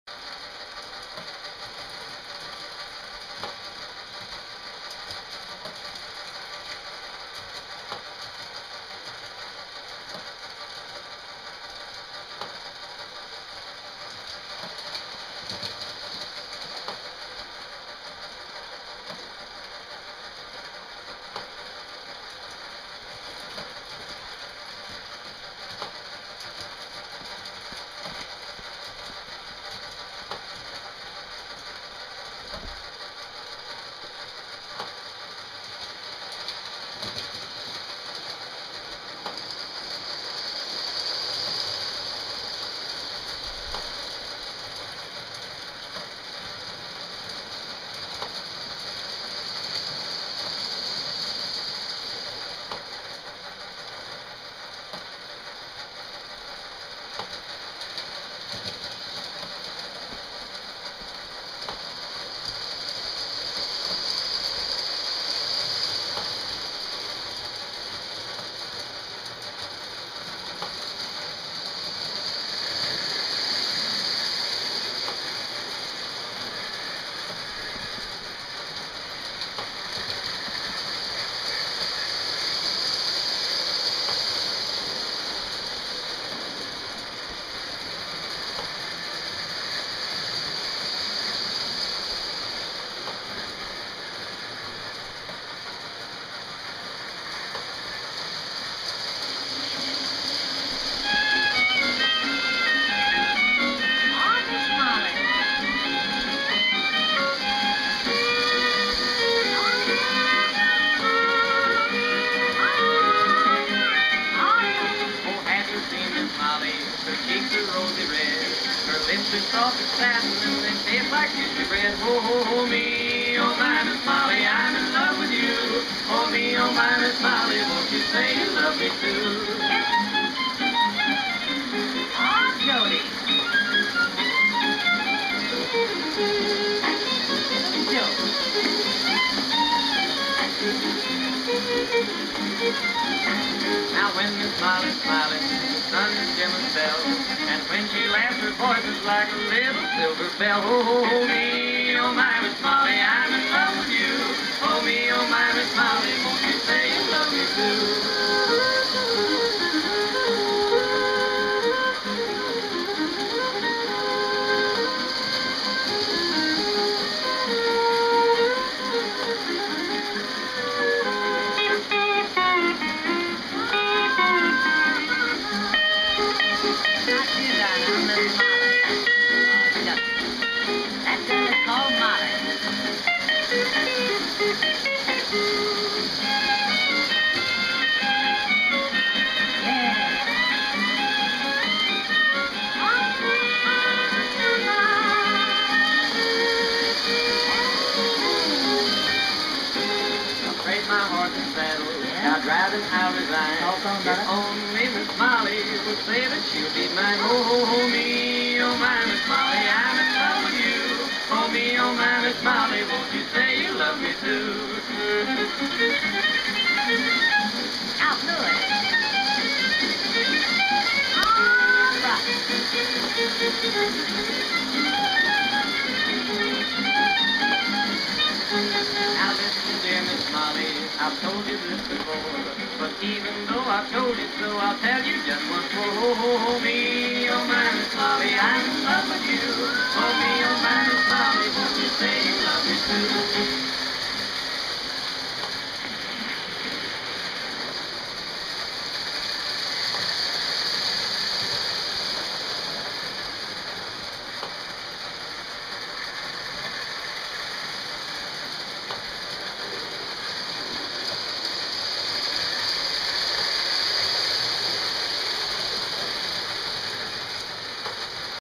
Marx Wlm. Crooks, Lionel, V&TRR, K-line Porter
Once a year in Porterville the town folk haul the old steam trains out of the barn, clean them up and run them around town. I was lucky to get prior notice from my friends Bossy and Bessie, so had my camera at the ready.
On the outside lower loop is a Lionel Virginia & Truckee railroad 'Dry Gulch Line' set which was a special service station set made in 1988. Running on the inside lower loop is a Marx WM Crooks set, with balloon stack and smoke, 1959-60. Up on the mesas is my familiar K-Line WP #6 Porter, made a few years back. The Porter is pulling a Lionel 6656 stock car and a Lionel AT&SF 9071 bobber caboose.